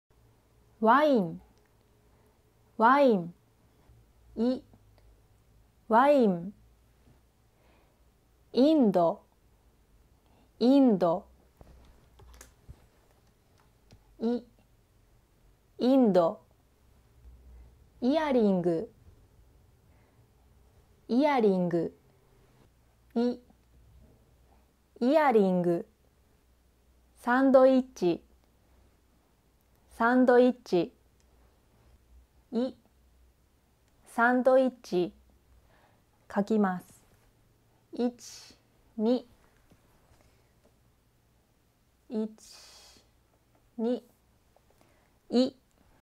#2【イ】katakana Short ver. 【JLPT N5】How to Read and Write Japanese Katakana
【イ】katakana Short ver. 【JLPT N5】How to Read and Write Japanese Katakana

チャンネルとうろくやおうえんうれしいです。

Word List
わたしはえいごがじょうずじゃないので、
さんこうていどでおねがいします。
Actually, my English is not good. So please use it as a reference.
ワイン：Wine
インド：India
イヤリング：Earrings
サンドイッチ：sandwich
かきます（書きます）：I will write